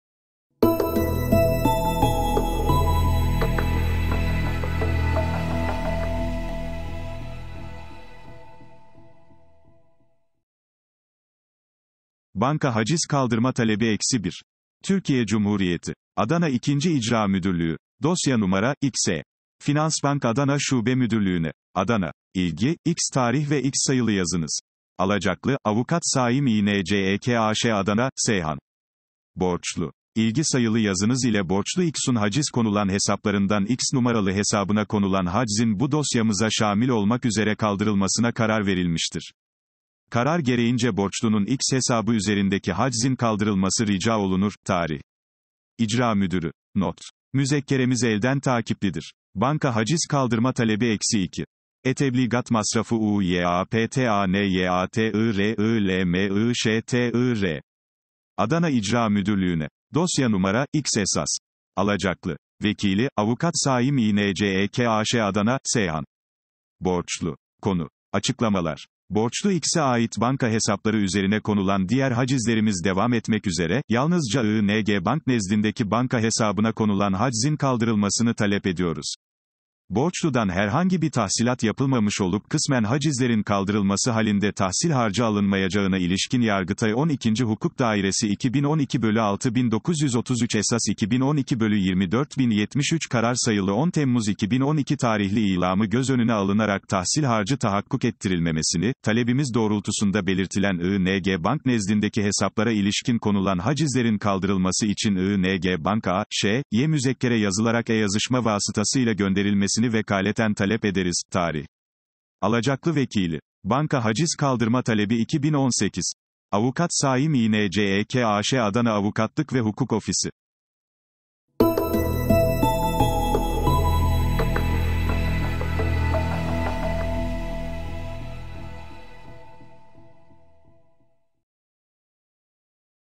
Bankadan Haciz Kaldırma Talebi Nasıl İstenir? - Avukat Saim İncekaş
Bankada kalkması gereken bir haciz uygulaması mı mevcut? İlgili bankayla iletişime geçerek haczin kaldırılmasını nasıl talep edeceğinizi sizler için açıkladık.